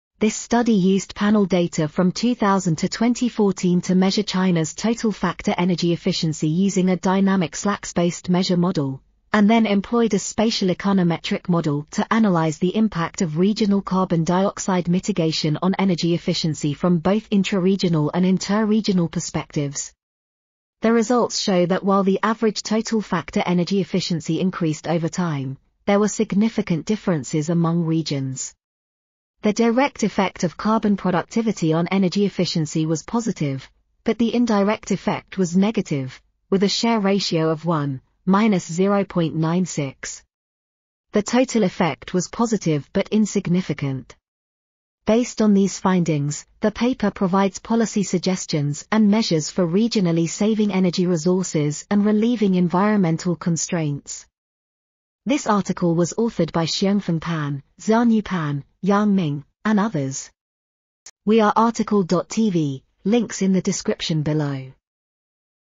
The effect of regional mitigation of carbon dioxide emission on energy efficiency in ... | RTCL.TV
### Keywords ###

### Article Attribution ###
Title: The effect of regional mitigation of carbon dioxide emission on energy efficiency in China, based on a spatial econometrics approach
Authors: Xiongfeng Pan, Xianyou Pan, Yang Ming ,and Jing Zhang
Publisher: Taylor & Francis Group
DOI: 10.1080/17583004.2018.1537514

### Video Timestamps ###
0:00:00 - Summary
0:00:55 - Title
0:01:01 - Tail
0:01:05 - End